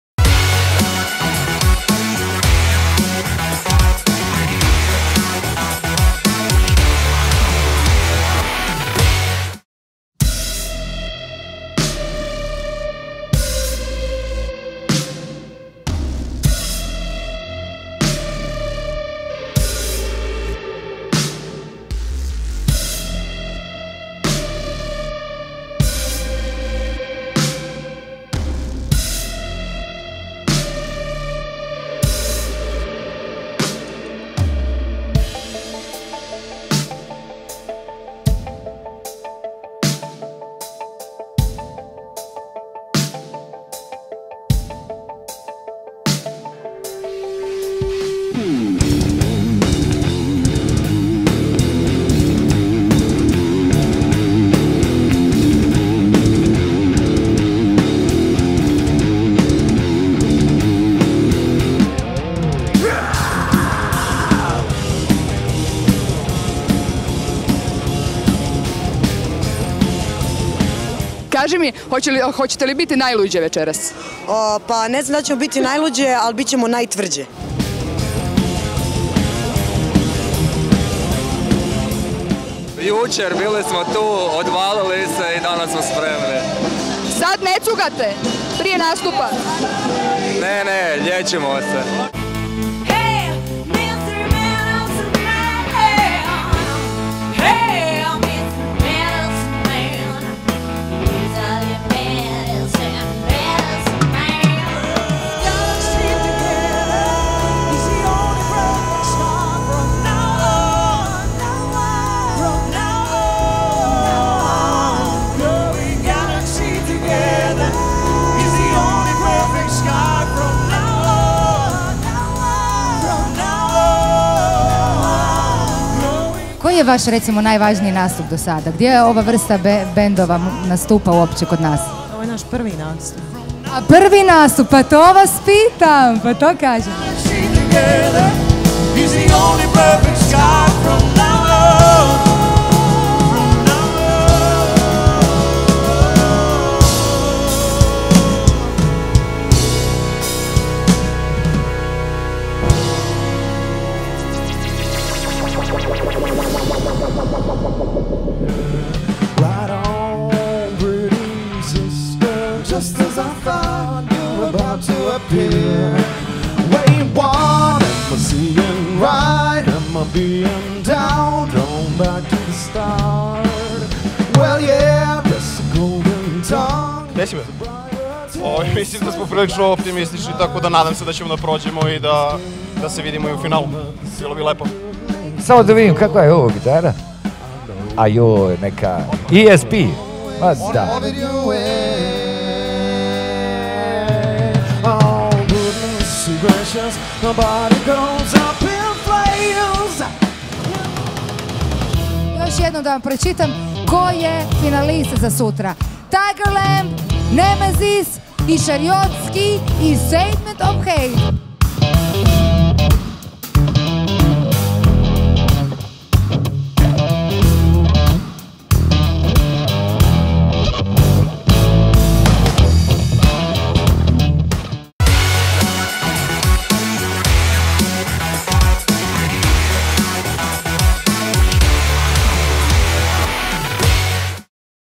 Demofest 10 - Drugo veče - Finalisti
Spisak bendova koji se u finalu takmiče za titulu najboljeg demo benda je kompletiran, a koji su to bendovi očarali žiri pogledajte u prilogu.